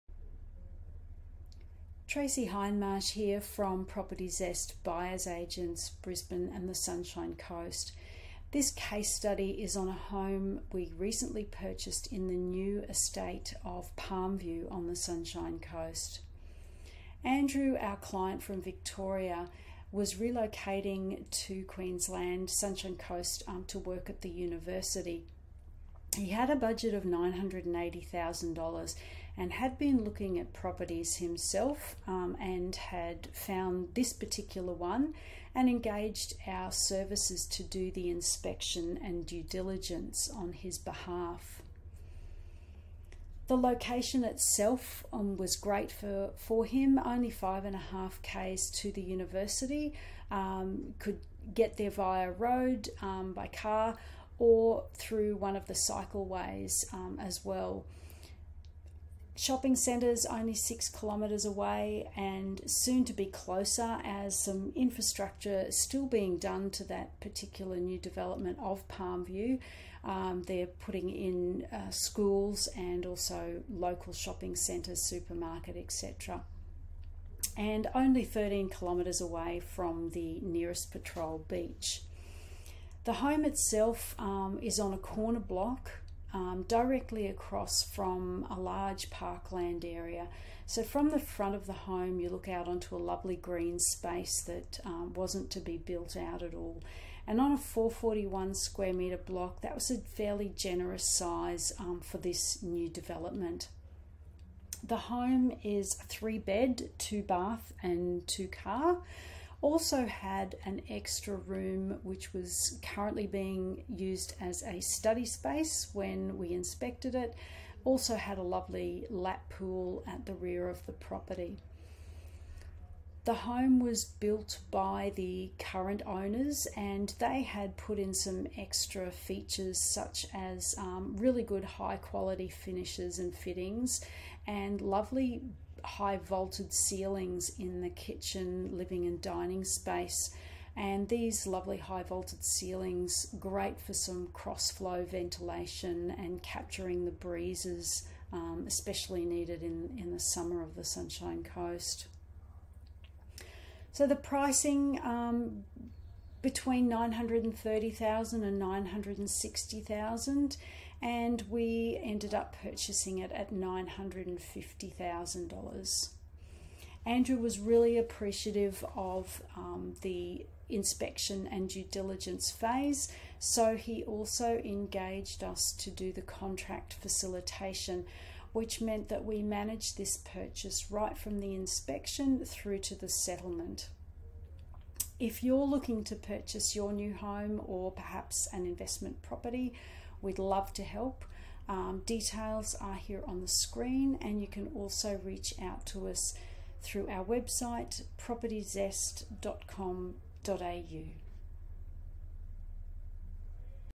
SUNSHINE COAST BUYERS AGENT CASE STUDY - PALMVIEW
- Sunshine Coast Buyers Agent and Brisbane Buyers Agent - This case study looks at a recent purchase in Palmview where we conducted an inspection and due diligence service.